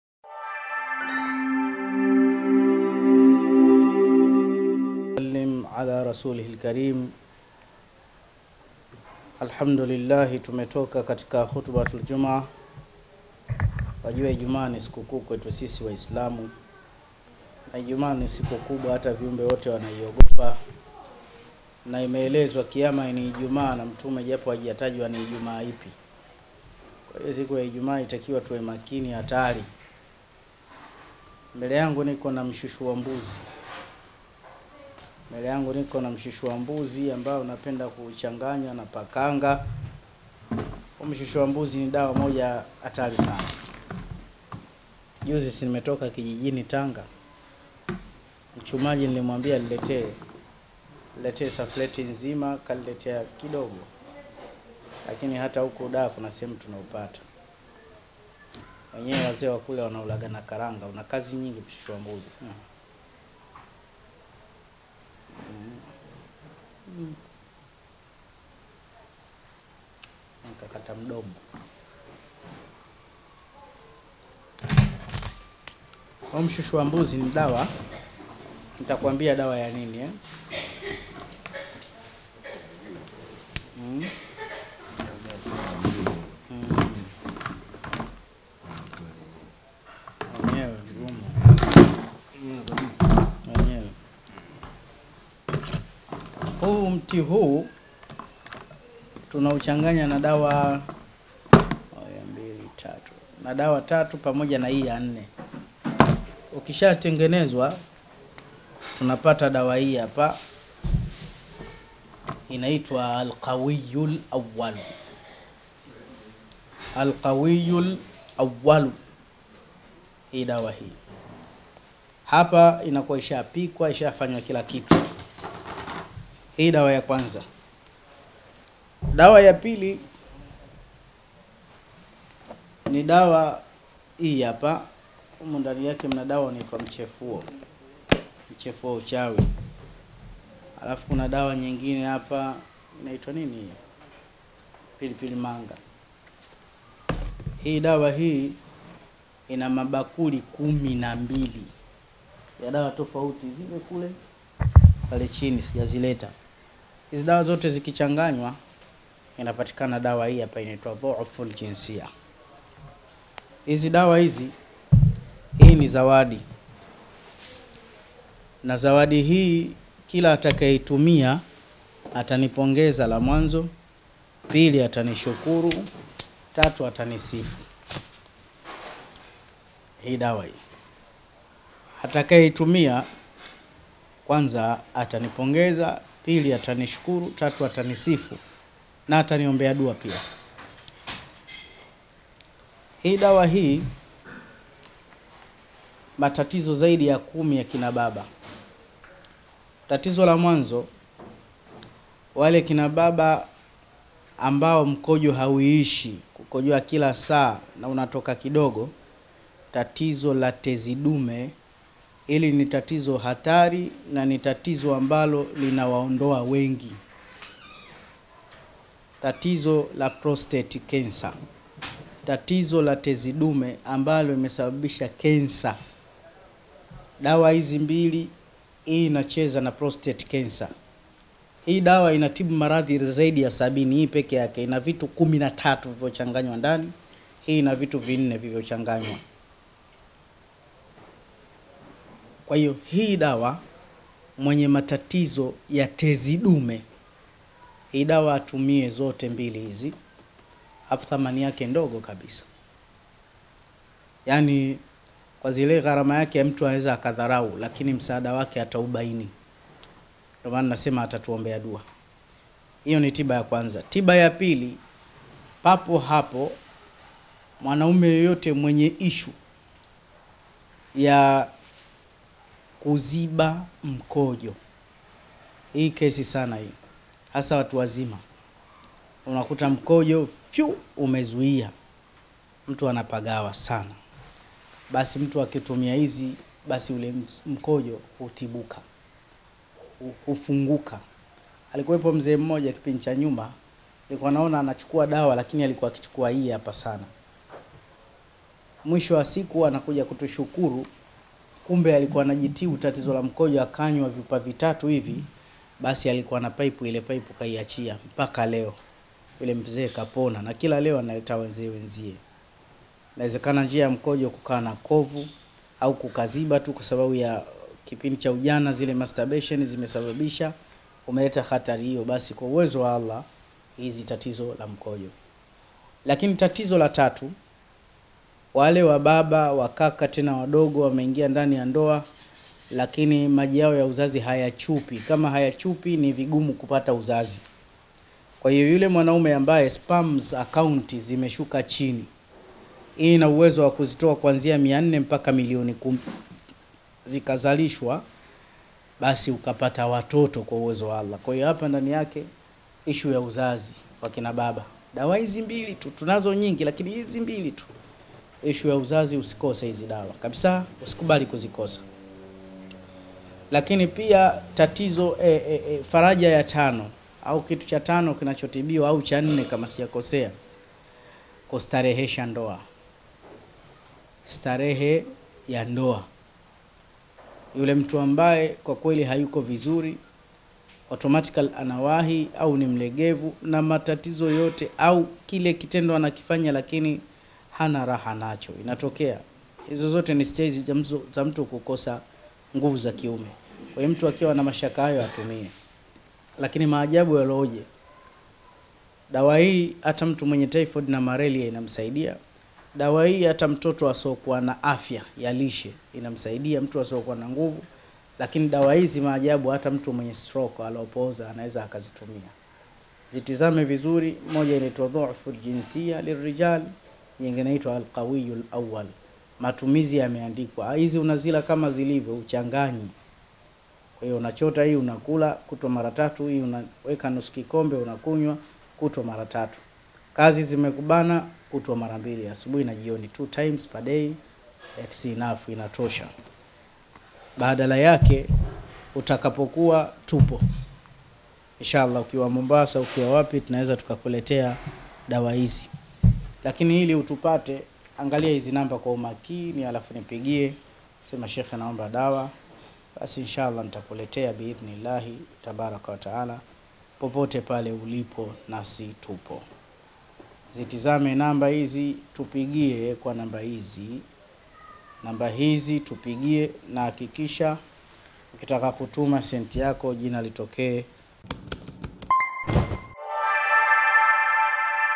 Chukua Zawadi Hii Kwa Matatizo 10 Ya Familia / Faida Ya Mshushua Mbuzi / Sheikh Othman Micheal
Mashaa Allah Sheikh Othman Micheal akielezea kwa uzuri na ubora wa hali ya juu Ya Chukua Zawadi Hii Kwa Matatizo 10 Ya Familia / Faida Ya Mshushua Mbuzi / Sheikh Othman Micheal

And
Thanks